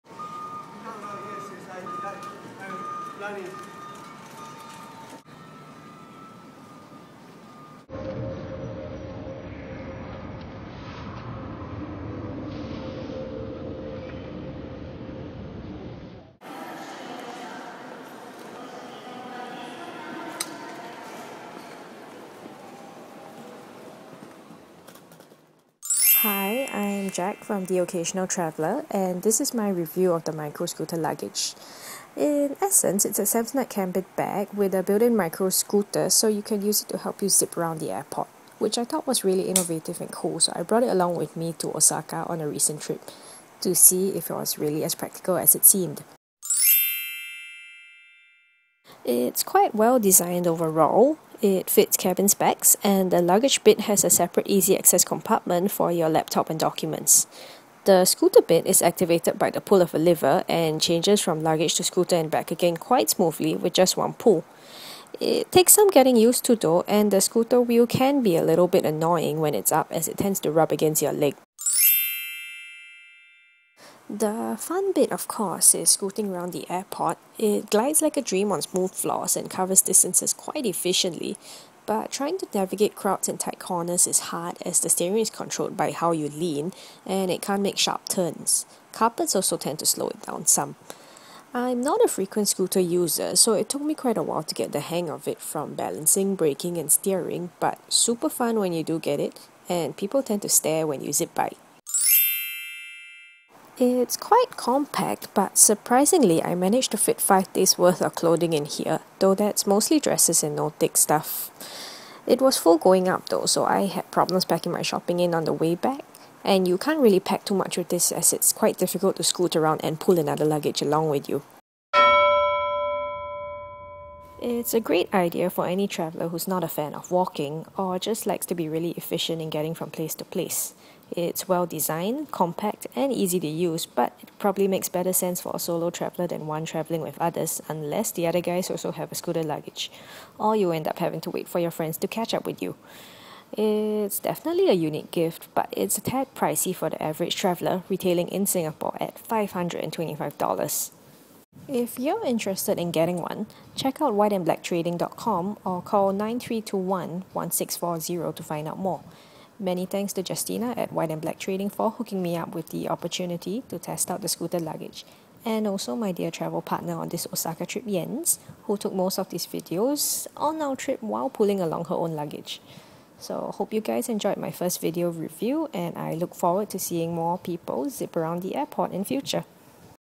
TOTreview - Micro Scooter Luggage
Jac from The Occasional Traveller takes the Micro Scooter Luggage, a hybrid cabin luggage cum scooter, out for a test run on a recent trip to Osaka, Japan.

More travel journals and inspirations over at theoccasionaltraveller.com